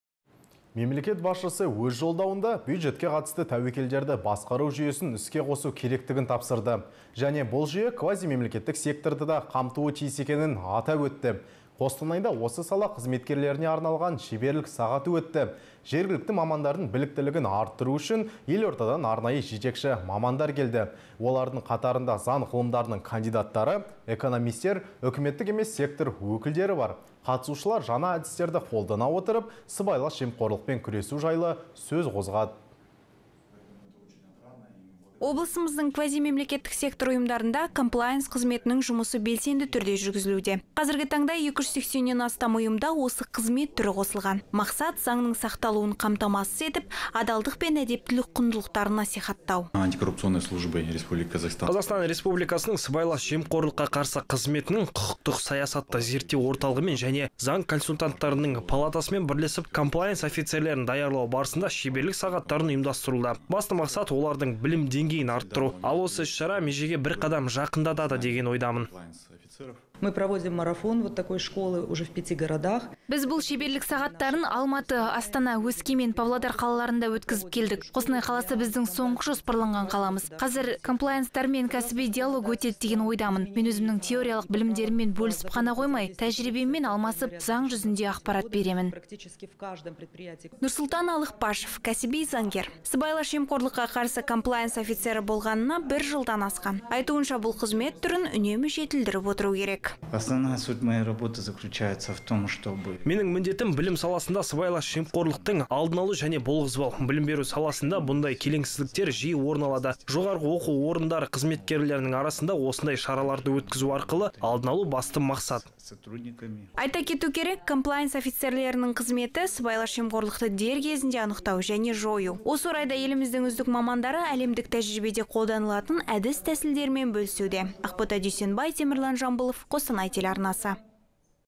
Жемқорлықпен күресудің тың тәсілдері
Мемлекет басшысы өз Жолдауында бюджетке қатысты тәуекелдерді басқару жүйесін іске қосу керектігін тапсырды. Және бұл жүйе квазимемлекеттік секторды да қамтуы тиіс екенін атап өтті. Қостанайда осы сала қызметкерлеріне арналған шеберлік сағаты өтті. Жергілікті мамандардың біліктілігін арттыру үшін елордадан арнайы жетекші мамандар келді. Олардың қатарында заң ғылымдарының кандидаттары, экономистер, үкіметтік емес сектор өкілдері бар. Қатысушылар жаңа әдістерді қолдана отырып, сыбайлас жемқорлықпен күресу жайлы сөз қозғады.